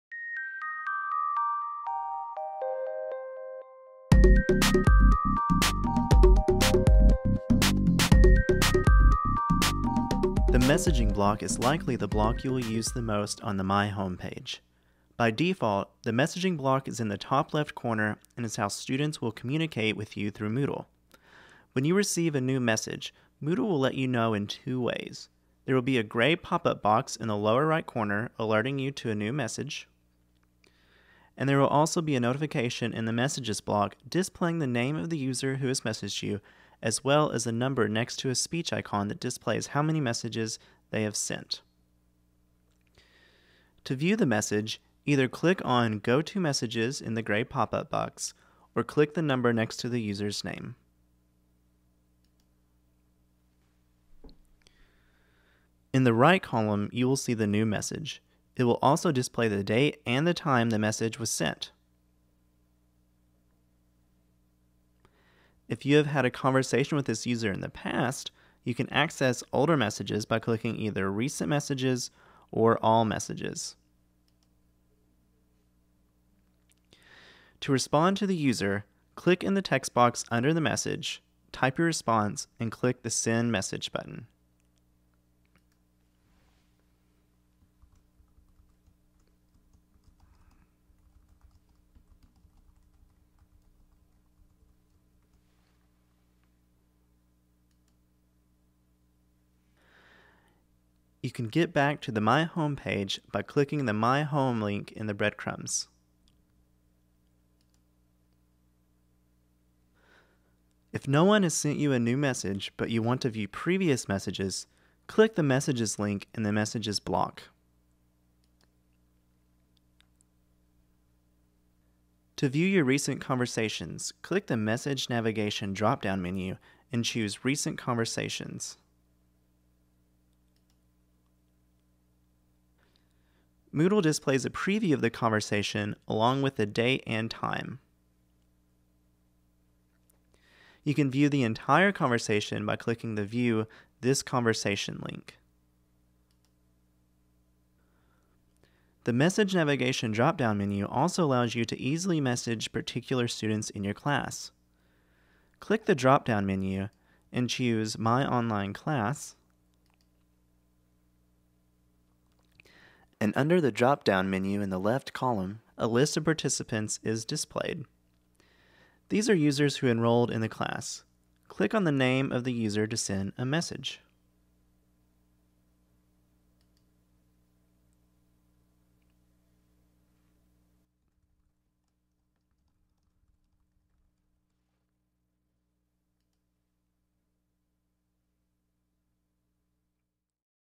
The Messaging Block -- Module 1
This is an introduction to the messaging block on Moodle at Black River Technical College.